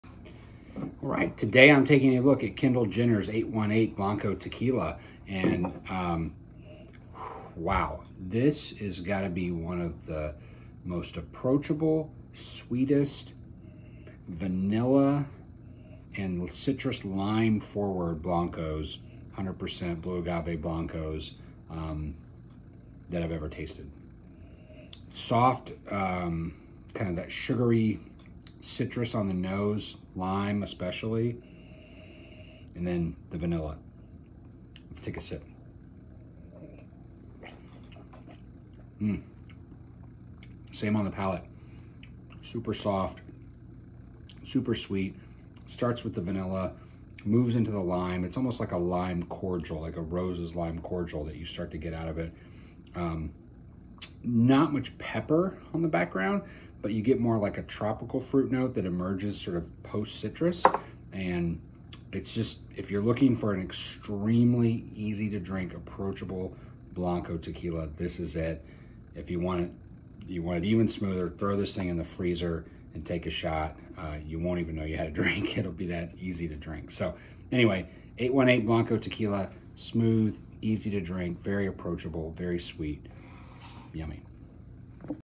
818 Tequila Blanco
818 Tequila Blanco is sugary sweet and insanely easy to drink with a sweet citrus mid-palate and a long, vanilla syrup finish. Kendall Jenner's 818 Blanco tequila is 100% blue agave, and one of the most approachable blancos on the market, which is the intent. Premium priced, but try it straight out of the freezer in a shot or a premium version of your favorite margarita.  🍯🌵🍹 Most Approachable Blanco